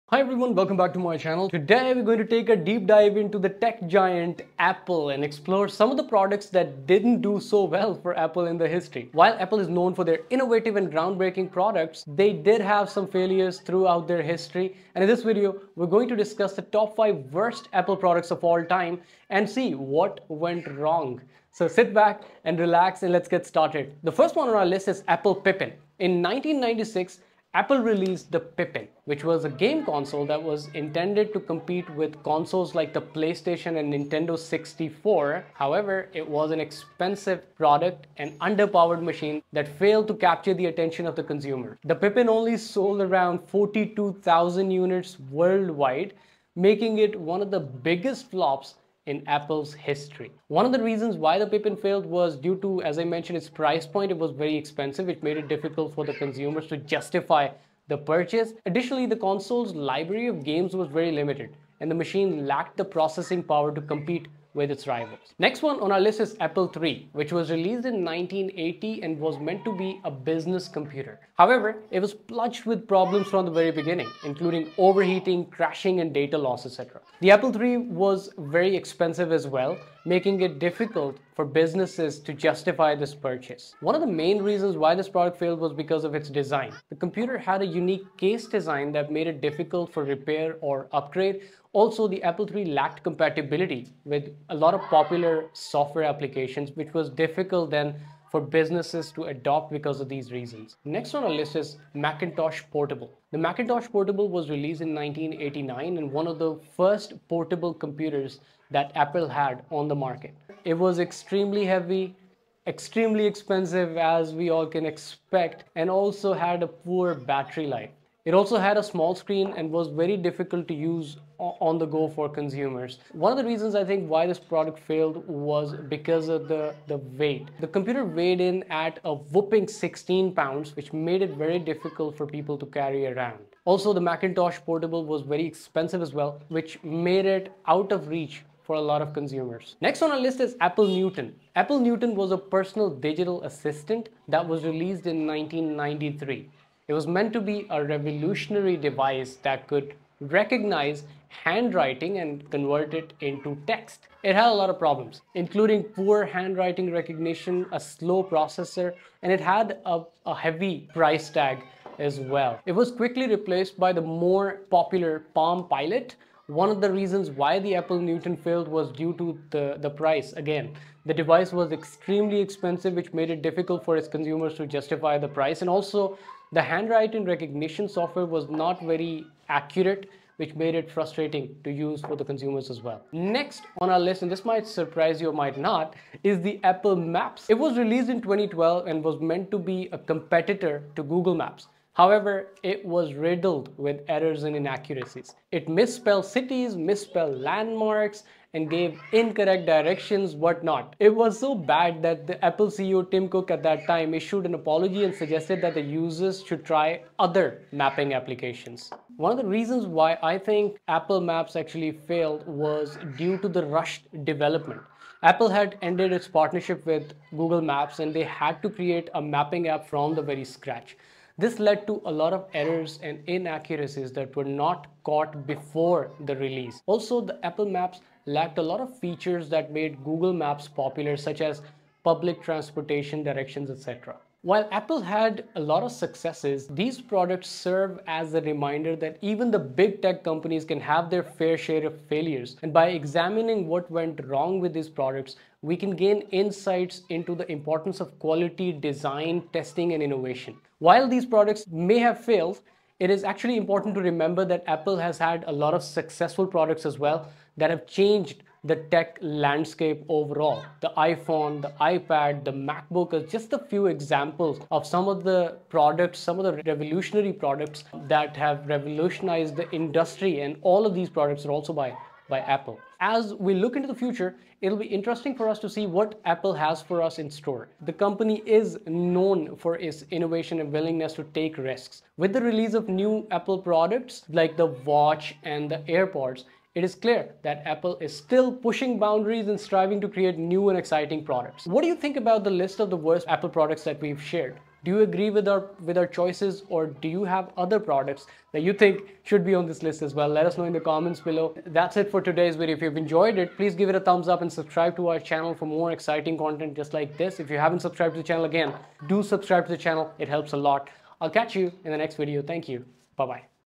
5 Worst Apple Products of All Time: Reasons Why They Failed
In this video, we're diving into the top 5 worst Apple products of all time and exploring the reasons why they failed. From awkward designs to technological flops, we'll take a deep dive into what made these products fall short of expectations. If you're an Apple enthusiast or just interested in learning more about the tech giant's history, you won't want to miss this video. So, grab some popcorn and join us as we explore the failures of some of Apple's most infamous products.